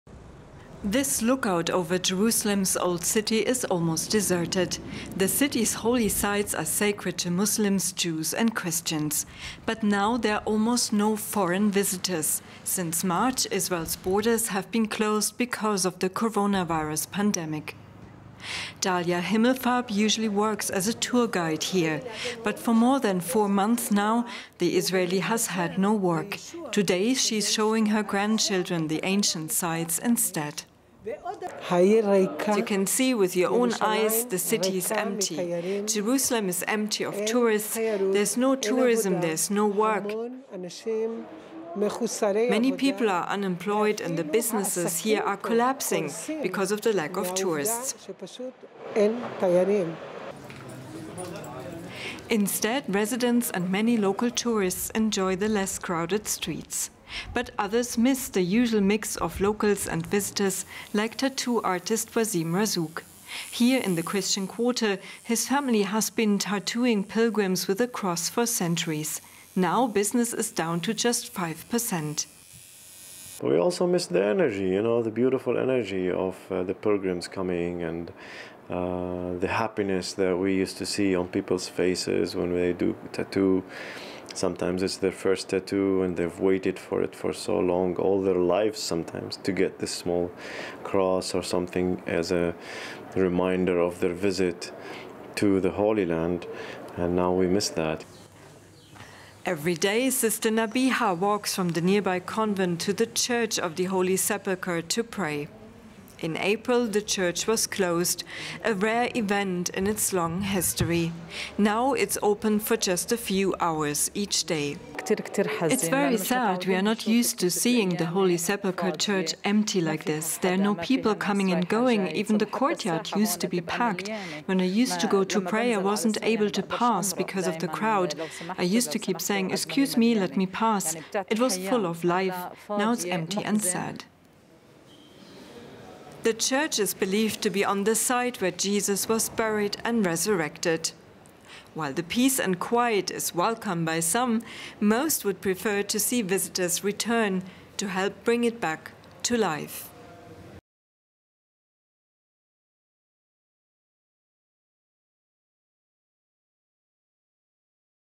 Coronavirus Israel: Lockdown rules leave Jerusalem empty of tourists | DW News
Israel was one of the first countries to impose a tough coronavirus lockdown - but also one of the first to ease restrictions. Now it's experiencing a second coronavirus wave, with more daily cases than ever before. It's again implemented strict measures. But that's worrying people in Jerusalem, where many livelihoods depend on catering to the pilgrims and visitors who have been part of city life for thousands of years.